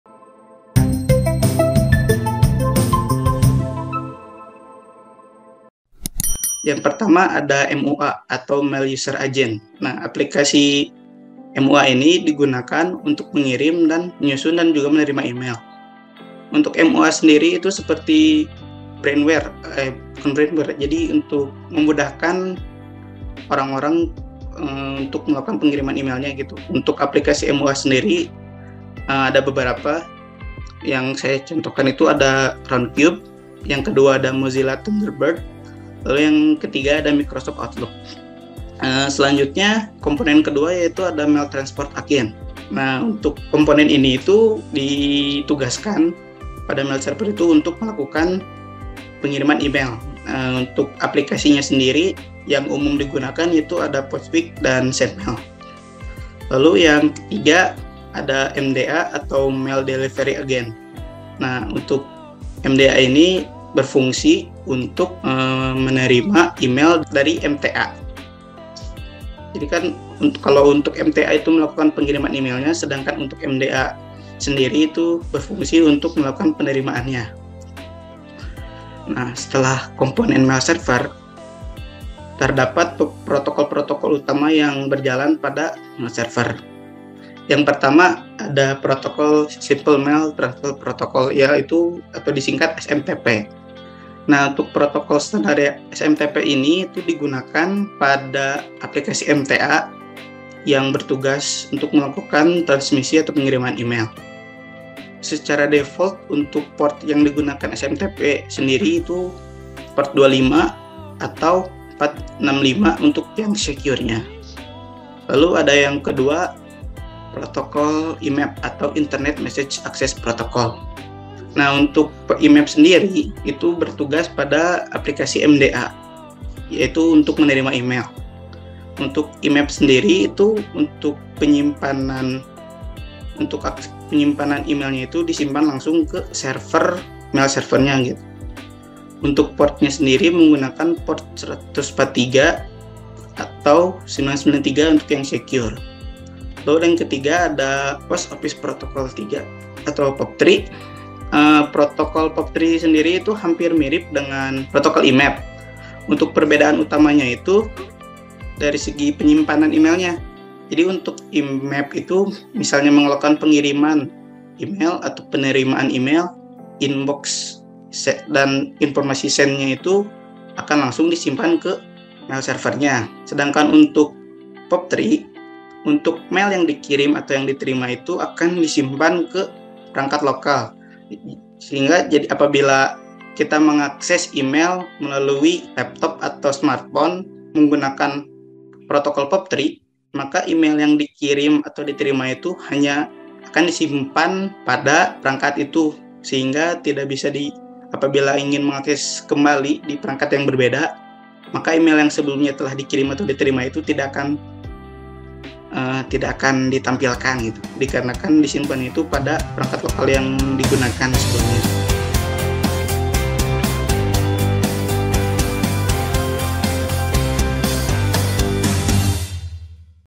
Kilat Minutes: Komponen dan Protokol Mail Server
"Sudah tahu belum ada beberapa komponen yang terdapat di Mail Server, atau protokol-protokol apa saja yang ada di Mail Server? Nah, untuk kamu yang belum tahu, kamu bisa simak video dari CloudKilat untuk jawabannya.

Ingin tahu penjelasan lebih lengkapnya? Kamu bisa langsung meluncur ke channel YouTube CloudKilat!